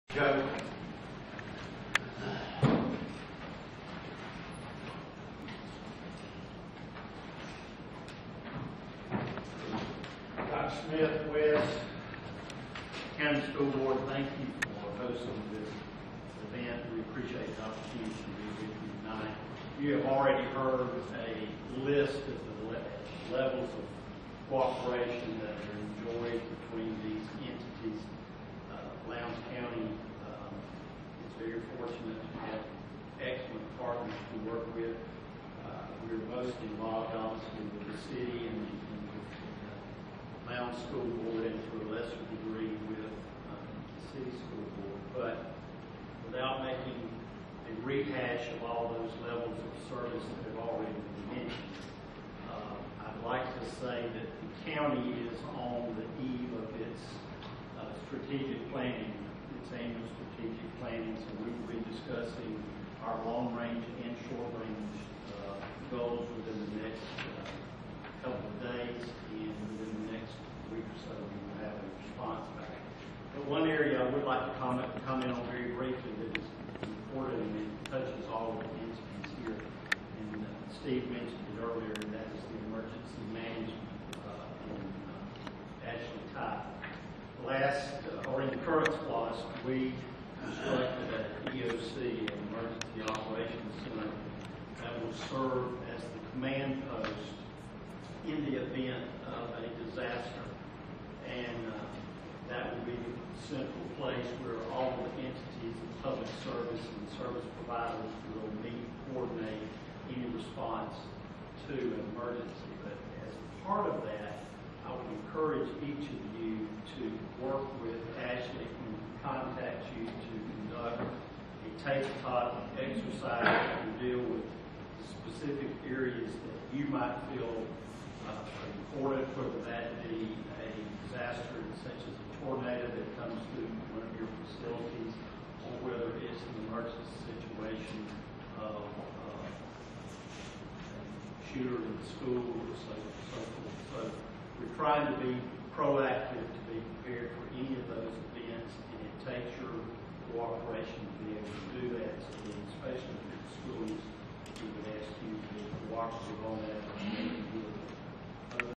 Emergency Management --Lowndes County Manager Joe Pritchard @ Joint Governments 2012-03-29
Communication, Expansion, Development, Strategy, Projects, Insight, Forecast
Valdosta-Lowndes Governmental Leadership Meeting, LCBOE, VBOE, LCC, VCC (Joint Governments)